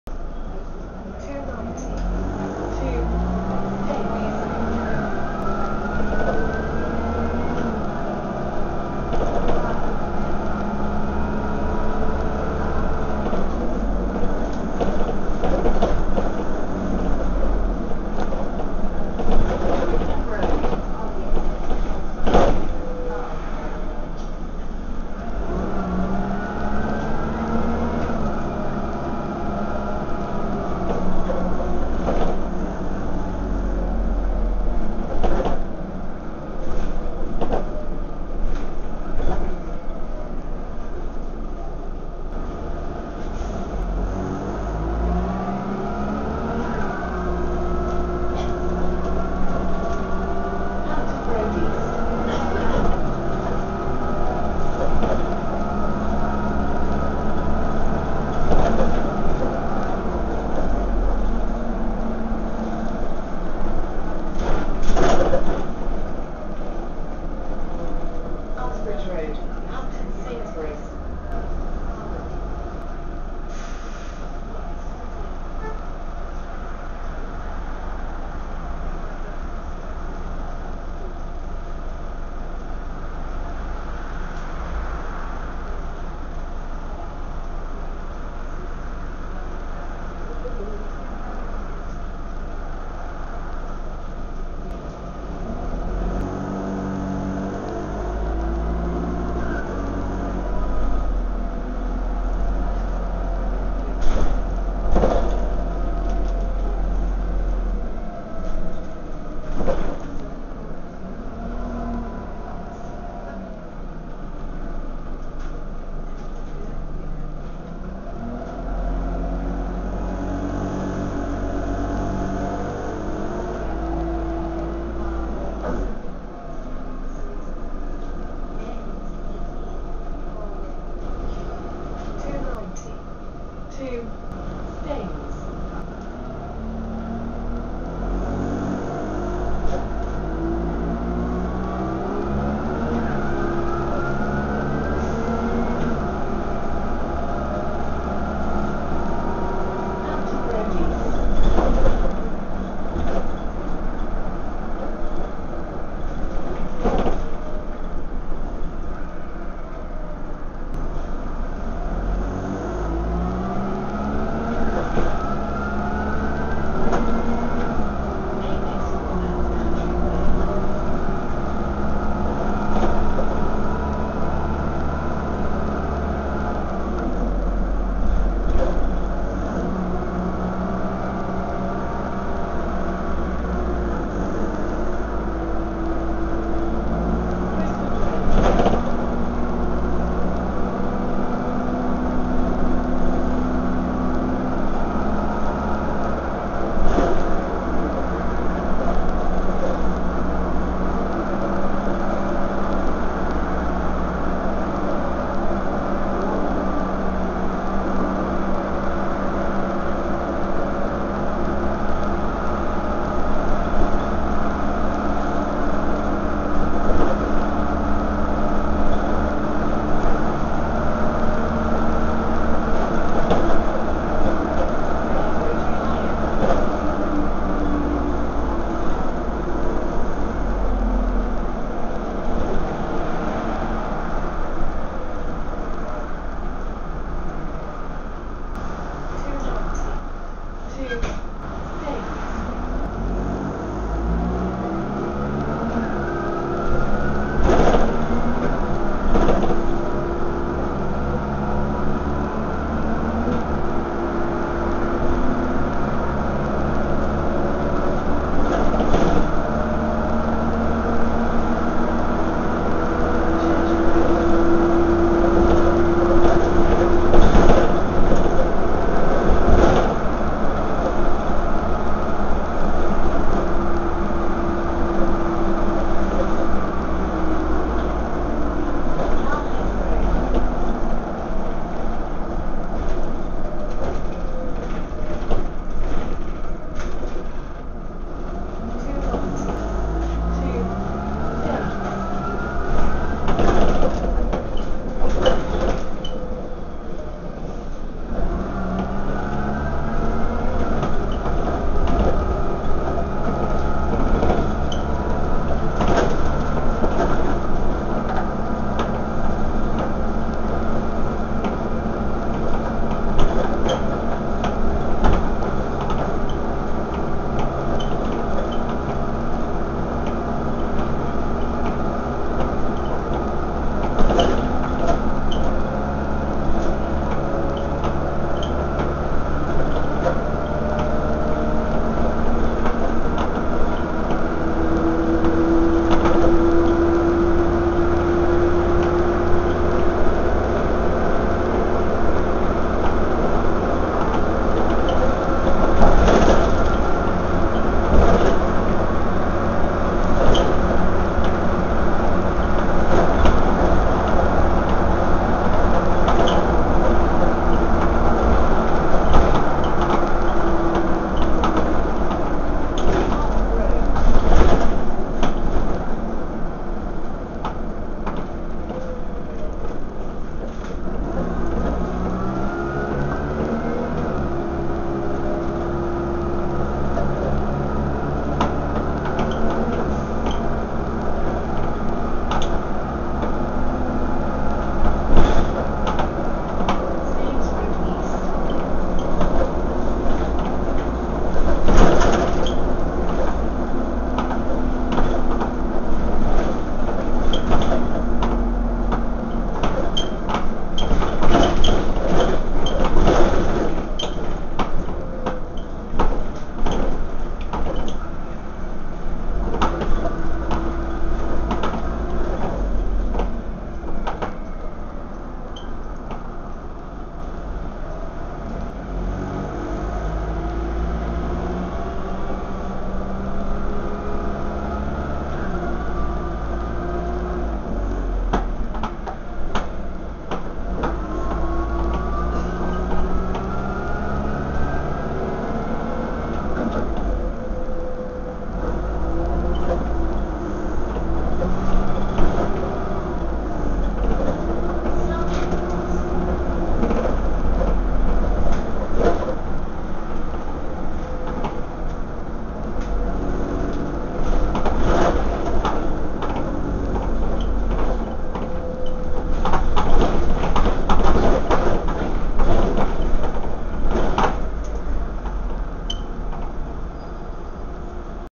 Journey On The 290 (DPS587,SN51TBO) Dennis Dart SLF 10.2m/Plaxton Pointer 2
DPS587 serving Route 290 in Hanworth/Sunbury.
Route 290: Twickenham - Staines.
Operated by London United - Fulwell (FW) Garage